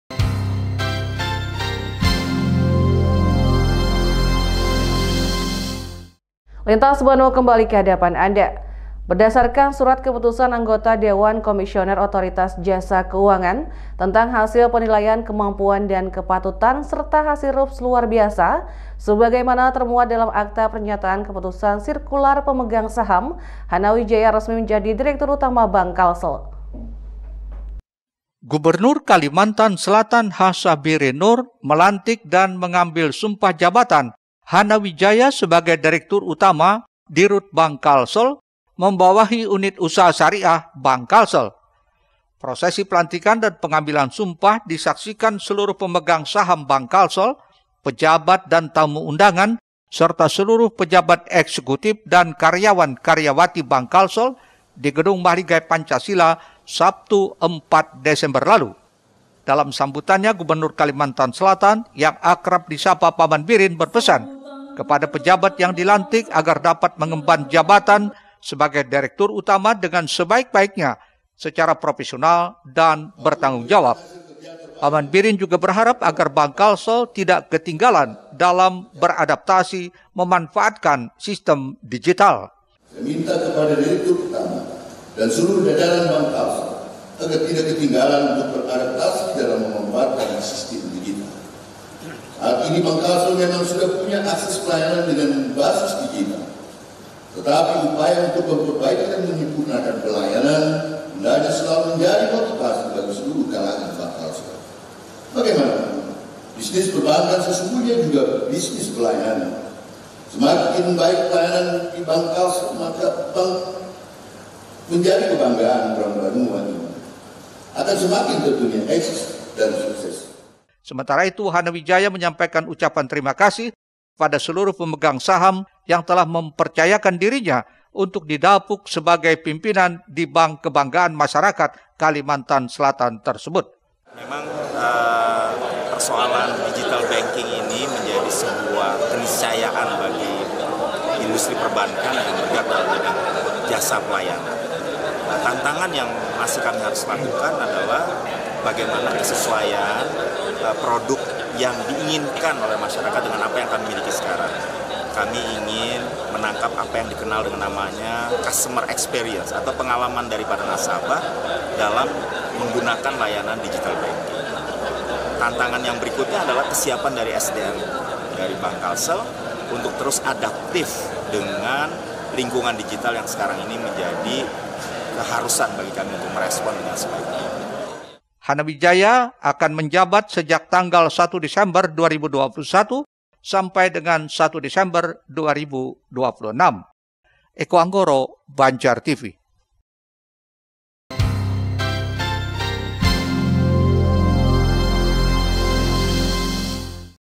HANAWIJAYA RESMI JABAT DIREKTUR UTAMA BANK KALSEL
Berdasarkan surat keputusan Anggota Dewan Komisioner Otoritas Jasa Keuangan tentang hasil penilaian kemampuan dan kepatutan serta hasil rups luar biasa, sebagaimana termuat dalam akta pernyataan keputusan sirkular pemegang saham, Hanawijaya resmi menjadi Direktur Utama Bank KALSEL.